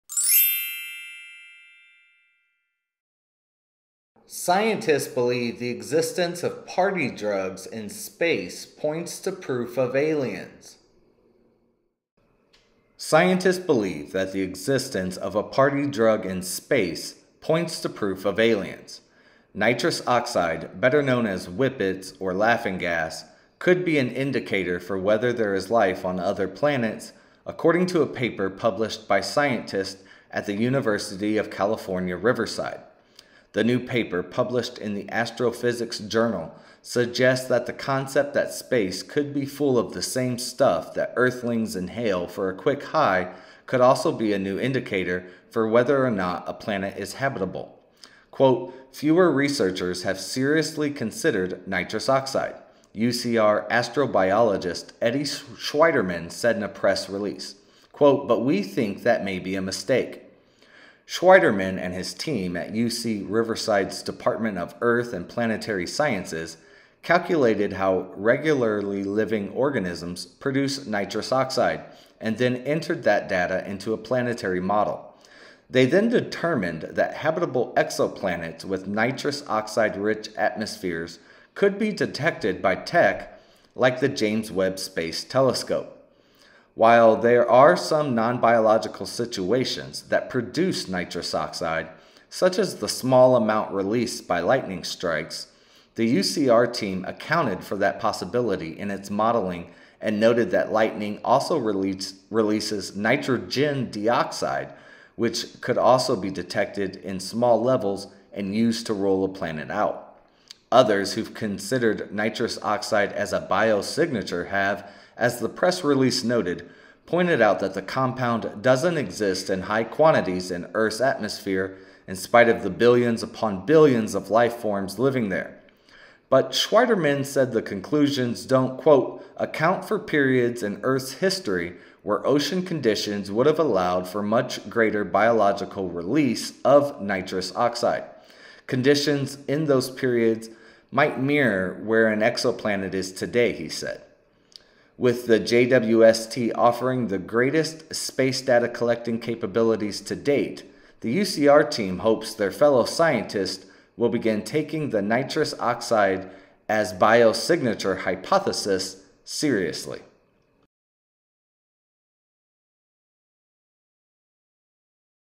Scientists believe existence of Nitrous Oxide in space points to proof of aliens
Scientists believe existence of party drugs in space points to proof of aliens
Scientists believe that the existence of a party drug in space points to proof of aliens. Nitrous oxide, better known as whippets or laughing gas, could be an indicator for whether there is life on other planets, according to a paper published by scientists at the University of California Riverside.

The new paper published in the Astrophysics Journal, suggests that the concept that space could be full of the same stuff that earthlings inhale for a quick high, could also be a new indicator for whether or not a planet is habitable.

"Fewer researchers have seriously considered nitrous oxide," UCR astrobiologist Eddie Schwieterman said in a press release, "but we think that may be a mistake."

Schwieterman and his team at UC Riverside's Department of Earth and Planetary Sciences calculated how regularly living organisms produce nitrous oxide, and then entered that data into a planetary model.

They then determined that habitable exoplanets with nitrous oxide-rich atmospheres could be detected by tech like the James Webb Space Telescope.